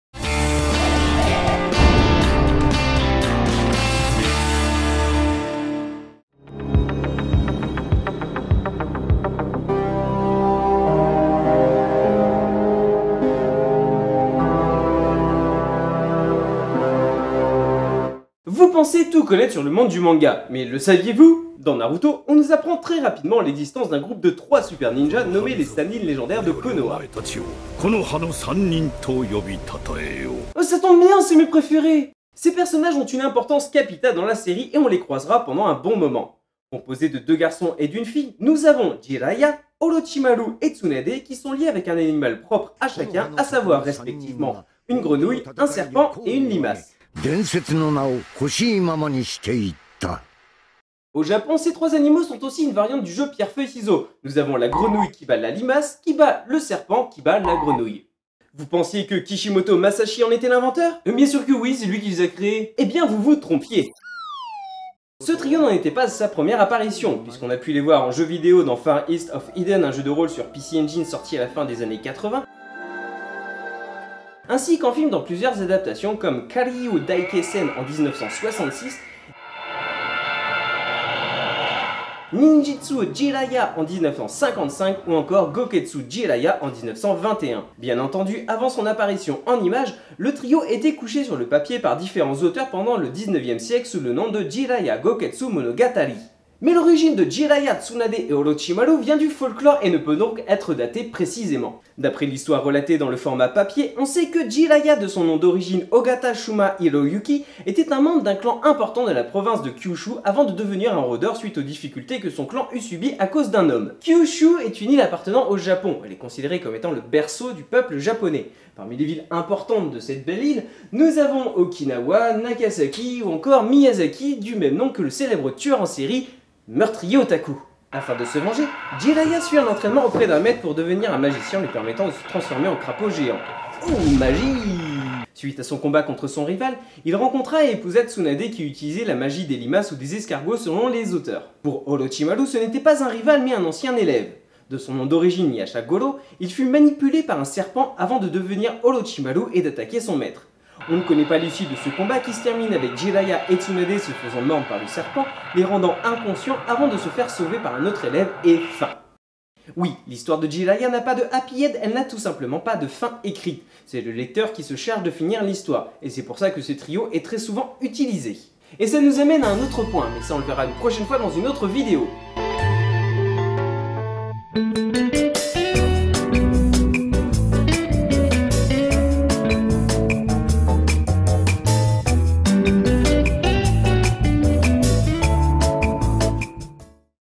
Les énigmes des mangas - Jiraiya, Tsunade et Orochimaru (Naruto)
L'énigme d'aujourd'hui nous plonge dans le monde de KISHIMOTO Masashi et sa série Naruto.
Qui est à l'origine des personnages de Jiraiya, Tsunade et Orochimaru ? Kishimoto, un lutin, un saucisson ? La réponse se trouve dans cette vidéo !

Vous pensez tout connaître sur le monde des mangas ? Mais le savez-vous ?